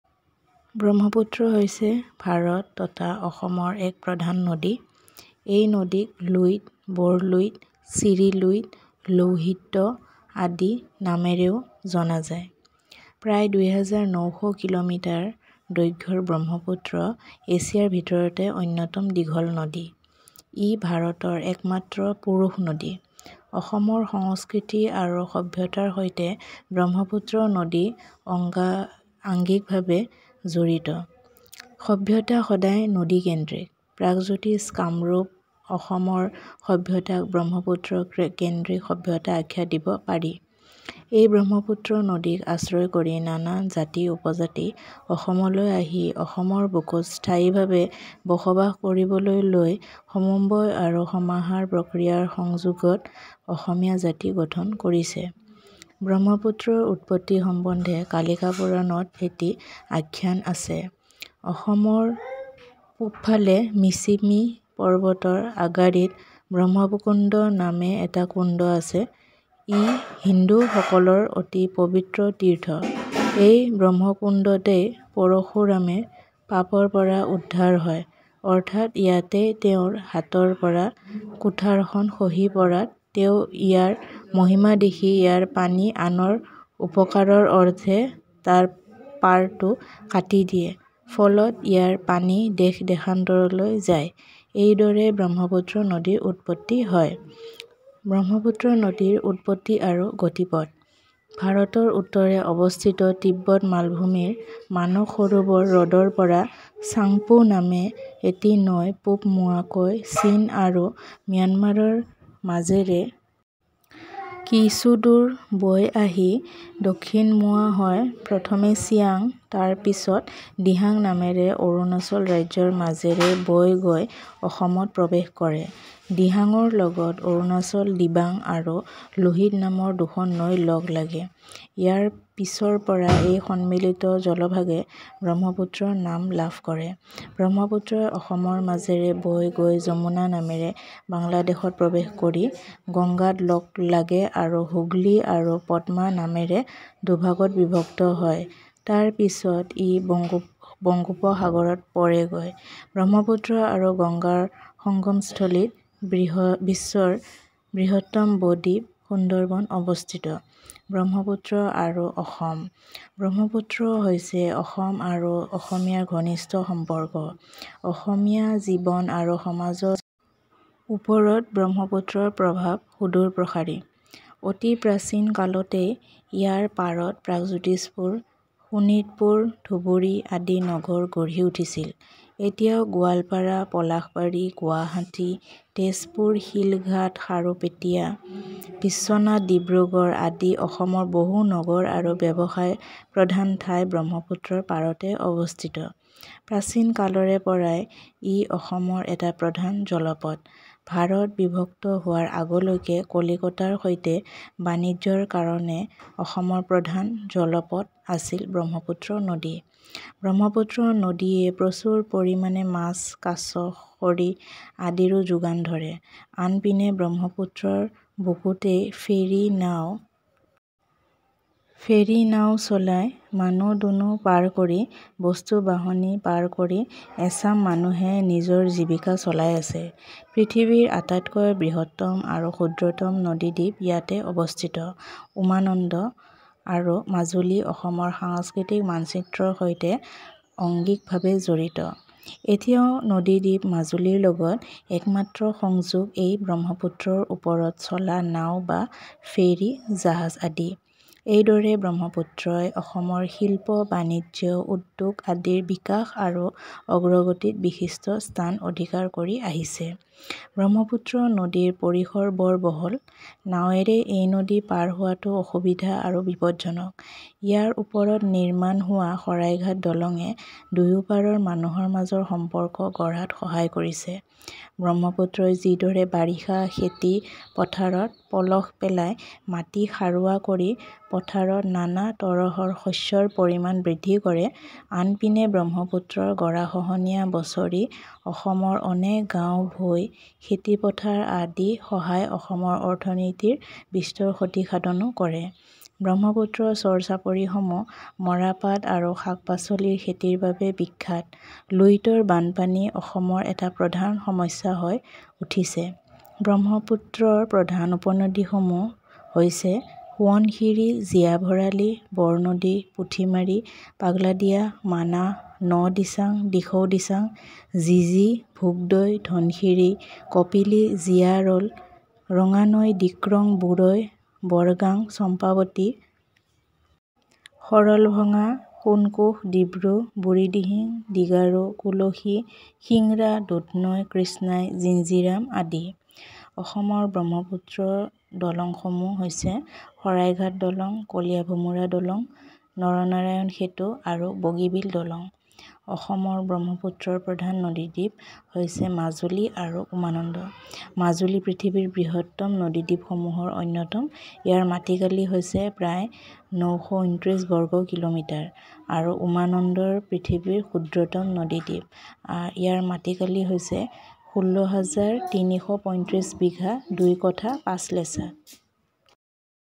Brahmaputra river / the history of Brahmaputra river in assamese / assam / indian male river
The Brahmaputra is a major river in India and Assam. The river is also known as the Luit, Barluit, Chiriluit, Lohitya, etc. The Brahmaputra is one of the longest rivers in Asia with a length of about 2,900 km. It is the only male river in India. The Brahmaputra River is inextricably linked to the culture and civilization of Assam. Civilization has always been river-centric. The Pragajyotisha-Kamrup-Assam civilization can be called a Brahmaputra-centric civilization. The Brahmaputra River is the source of the Assamese nation. The Brahmaputra River is the source of the Assamese nation. The Brahmaputra River is the source of the Assamese nation.

There is a legend in the Kalika Purana about the origin of the Brahmaputra. There is a well called Brahmkund at the foot of the Michimi Mountains in the eastern part of Assam. It is a very holy pilgrimage site for Hindus. It is in this Brahmkund that Parashurama is saved from sin, that is, it is here that the ax falls from his hand and he sees its glory and cuts off its banks for the benefit of others. As a result, its water goes abroad. This is how the Brahmaputra River originates

origin and course
From Lake Manas in the Tibetan Plateau in northern India, the Changpo flows eastwards through China and Myanmar, flows southwards, first through the Chiang and then through the Dihang state of Arunachal Pradesh and enters Assam. The Dihang is joined by two rivers in Arunachal Pradesh, the Dibang and the Lohit. The combined watershed was named the Brahmaputra. The Brahmaputra flows through Assam, enters Bangladesh as the Jamuna, joins the Ganges and divides into two rivers, the Hooghly and the Padma. It then flows into the Bay of Bengal. The world's largest bay island, the Sundarbans, is located at the confluence of the Brahmaputra and Ganges.

Brahmaputra and Assam
Assam and Assamese have a close relationship with the Brahmaputra. The Brahmaputra has a far-reaching impact on Assamese life and society. In ancient times, cities like Pragjyotishpur, Sonitpur and Dhubri were built on its banks. There are still many cities and commercial places in Assam like Goalpara, Palashbari, Guwahati, Tezpur, Shilghat, Kharupetia, Bishwanath, Dibrugarh etc. located on the banks of the Brahmaputra. It has been a major waterway in Assam since ancient times. Until the partition of India, the Brahmaputra River was the main waterway of Assam for trade with Calcutta.

The Brahmaputra River also provides abundant fish, fish, firewood etc. There are many people who earn their living by driving ferries, crossing people and goods on the banks of the Brahmaputra. It is home to the largest and smallest river islands in the world. Uma Nanda(peacock island) and Majuli are inextricably linked to the cultural map of Assam. The only connection to the river island of Majuli is still by boat or ferry on the Brahmaputra. Thus, the Brahmaputra occupies a prominent place in the development and progress of industry, commerce, industry, etc. in Assam.

The Brahmaputra River is very wide. Crossing this river by boat is difficult and dangerous. The Saraighat Bridge built on it has helped to build ties between the people on both sides. The Brahmaputra floods cause huge damage to the economy of Assam by eroding many villages, fields etc. every year. The Brahmaputra floods cause huge damage to the economy of Assam by eroding many villages, fields etc. every year. The char-chaparis of the Brahmaputra are famous for cotton and vegetable cultivation. Luit flooding has become a major problem in Assam.

Major tributaries of the Brahmaputra in Assam

Bridges on the Brahmaputra in Assam
The Saraighat Bridge, Kaliabhomora Bridge, Nar Narayan Bridge and Gigibil Bridge

It is the main river island of the Brahmaputra in Assam
Majuli and Uma Nanda. Majuli is one of the largest river islands in the world. (area about 929 sq km) and Umanad is the smallest river island in the world (area 16.335 bighas 2 katha 5 lechas).